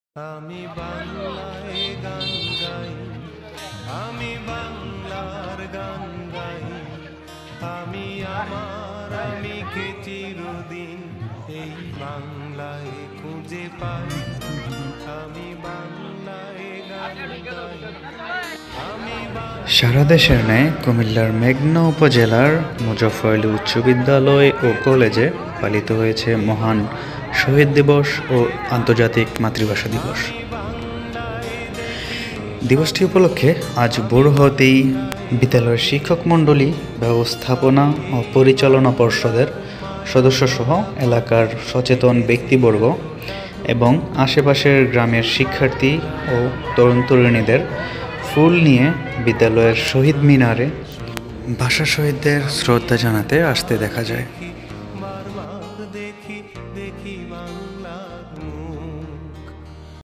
মুজাফফর আলী উচ্চ বিদ্যালয় ও কলেজে মহান শহীদ দিবস ও আন্তর্জাতিক মাতৃভাষা দিবস।
সাড়াদেশের ন্যায় কুমিল্লার মেঘনা উপজেলার মুজাফফর আলী উচ্চ বিদ্যালয় ও কলেজে পালিত হয়েছে মহান শহীদ দিবস ও আন্তর্জাতিক মাতৃভাষা দিবস।